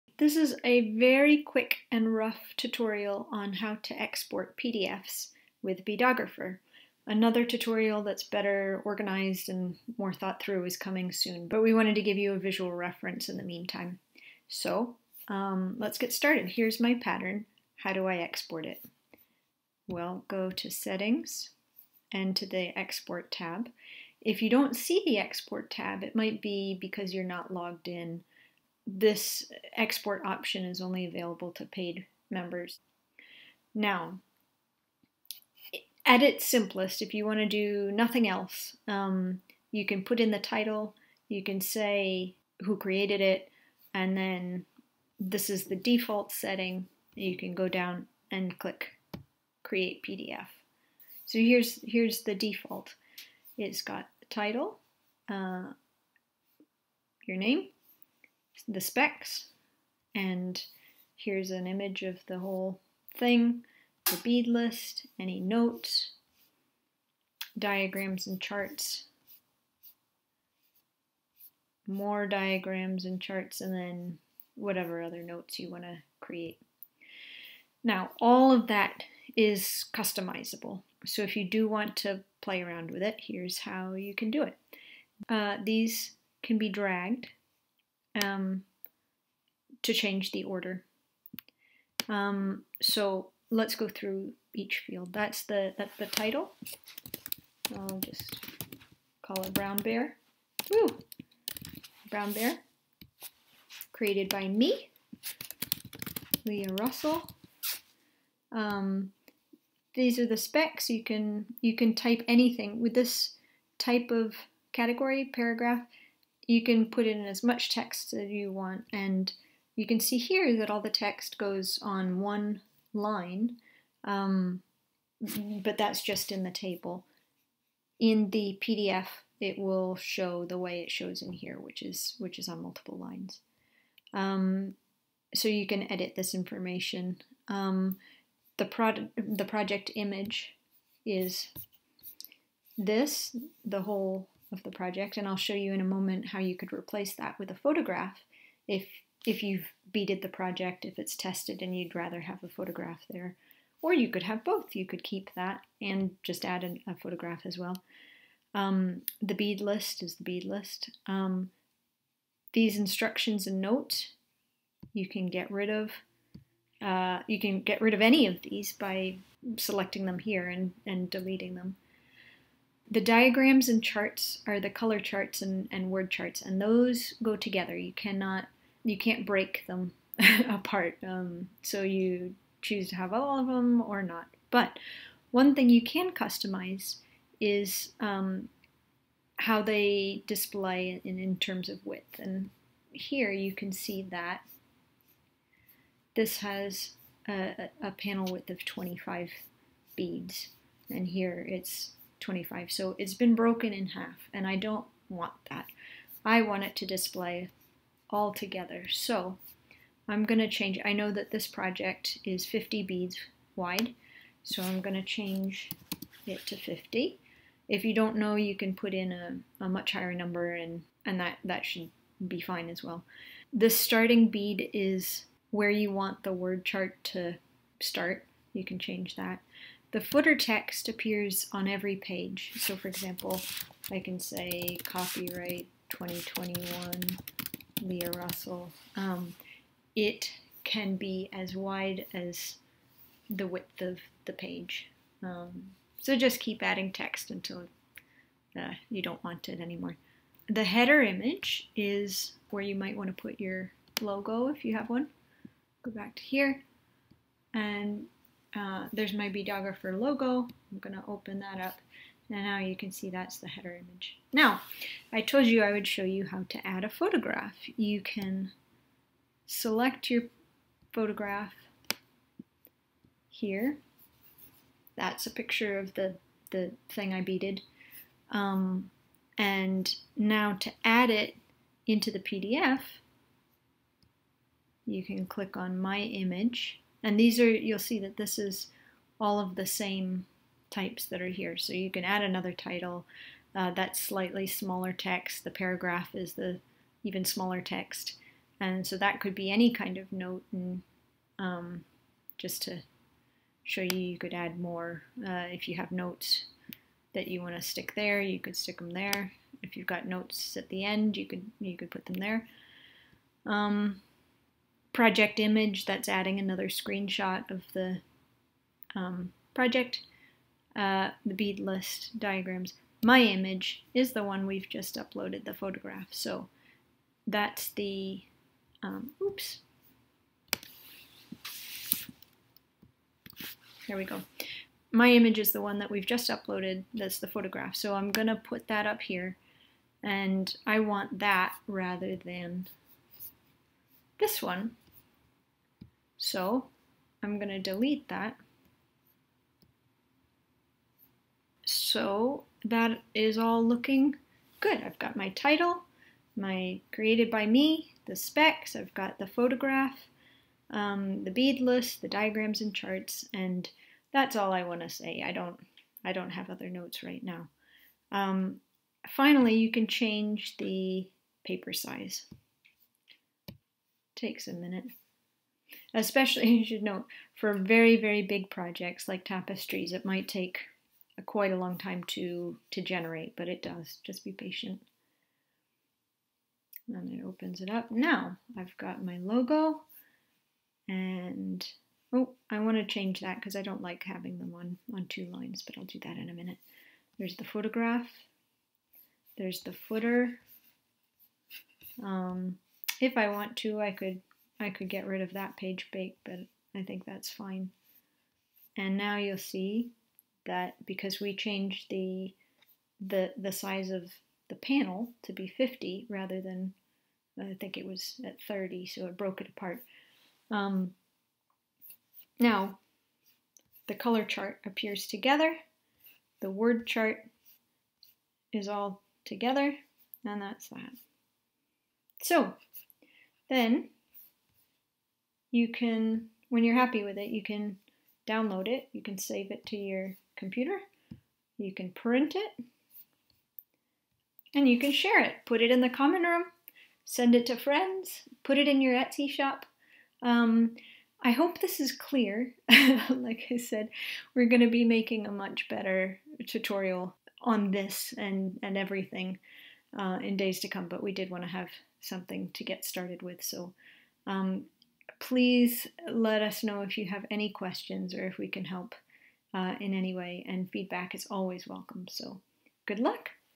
ARCHIVED: Overview on exporting pdfs -- temporary tutorial
Now that Version 2 of Beadographer is up and live, this is a quick-fix tutorial on how to export pdfs of your patterns. We'll make a nicer video as soon as we can, but hopefully this should answer some questions you might have about how exports work in the meantime.